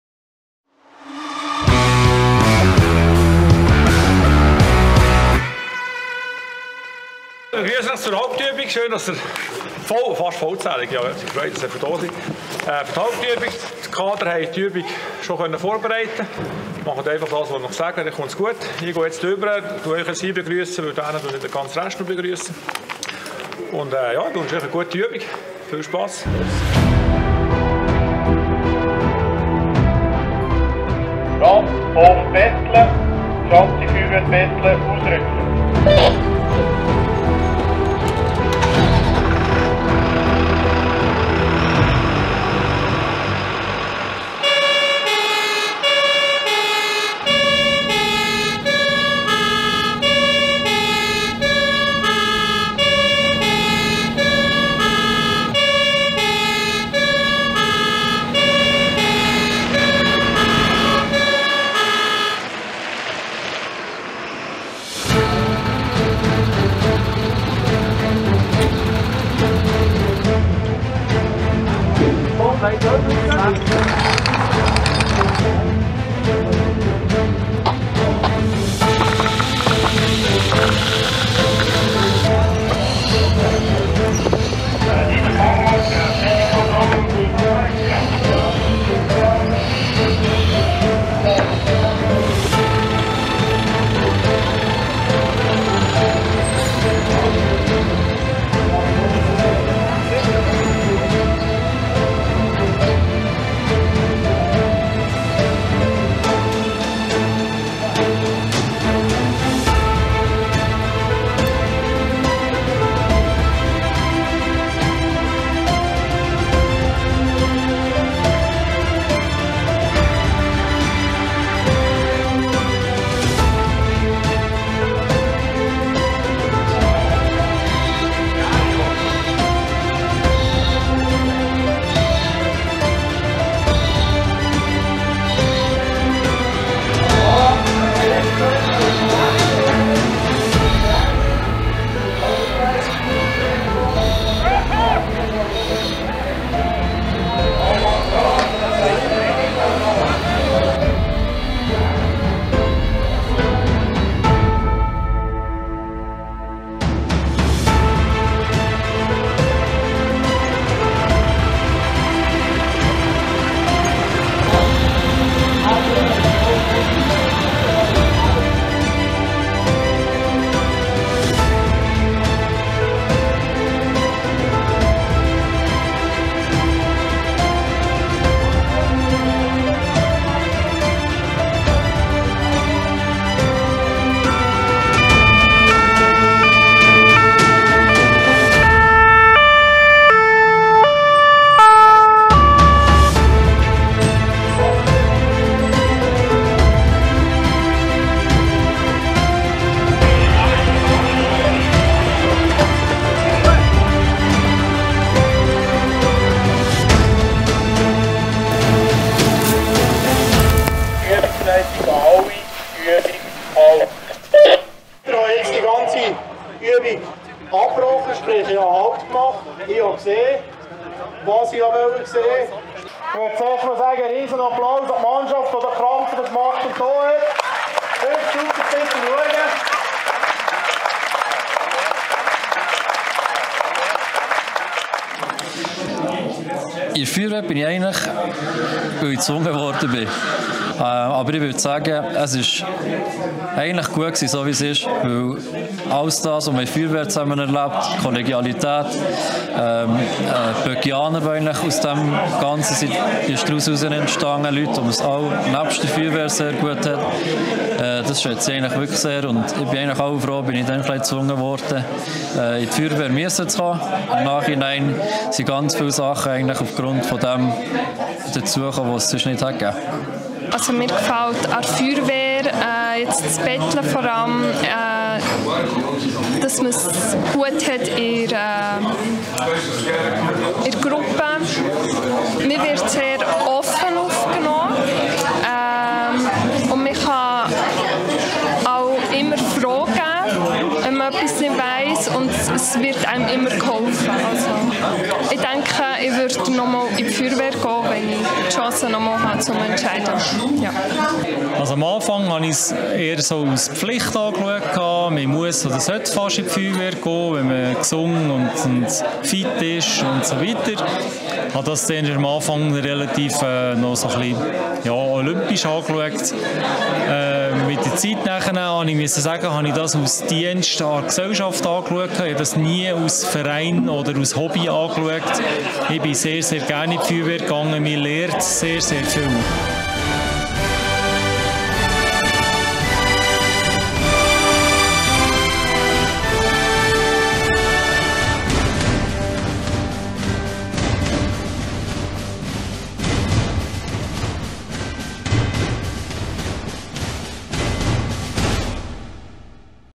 Feuerwehr Bettlach, Hauptübung 2022
Die Feuerwehr Hauptübung in Bettlach wurde 2022 zusammen mit den Feuerwehren Grenchen, Biebrist und Selzach durchgeführt. Eine monumentale Show!